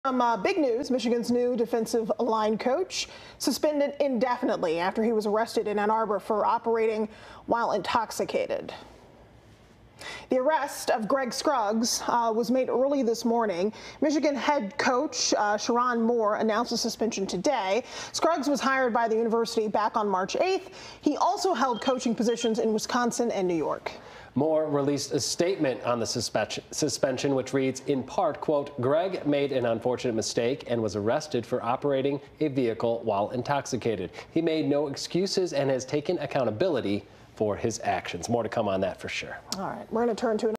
Greg Scruggs, Michigan's defensive line coach, arrested in Ann Arbor
Scruggs was arrested March 16, just 8 days after being hired, for allegedly operating a vehicle while intoxicated.